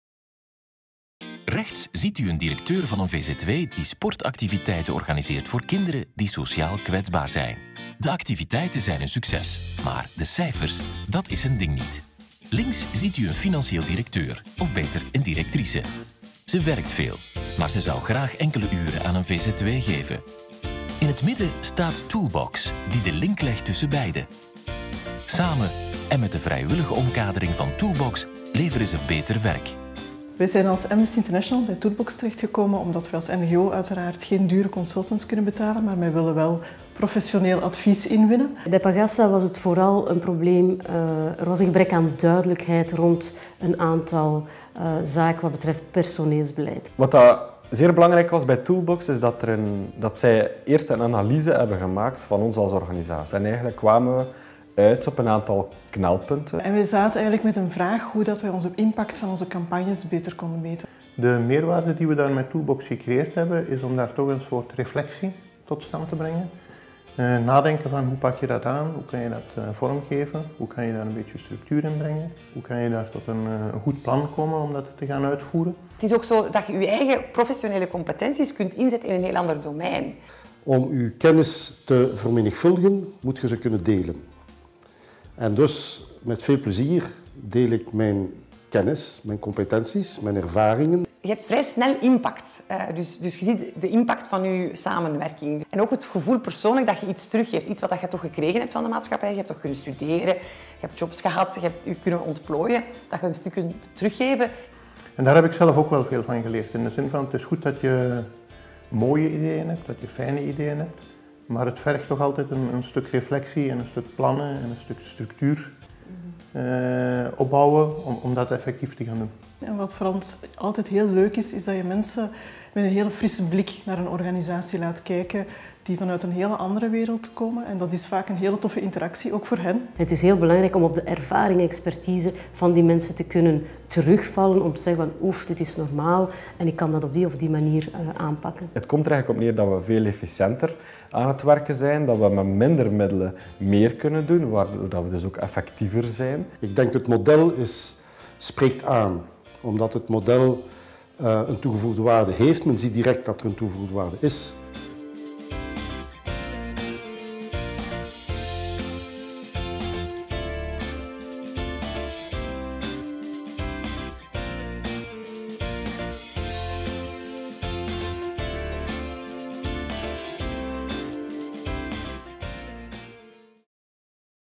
ToolBox - Bridging human capital (Nederlands)
ToolBox is een vzw die het management van verenigingen wil ondersteunen en verbeteren door, op aanvraag, expertise ter beschikking te stellen.

Een 90-tal maatschappelijk betrokken professionals zijn lid van ToolBox en stellen pro bono hun expertise en kennis op het vlak van strategie, HR, marketing en communicatie, ICT, financiële of juridische zaken ter beschikking van vzw's die dank zij deze coaching zich verder kunnen professionaliseren.

ToolBox brengt vraag en aanbod samen, selecteert gemotiveerde vrijwilligers en vzw's en biedt hen de nodige ondersteuning en methodologie aan ter vervulling van hun opdracht.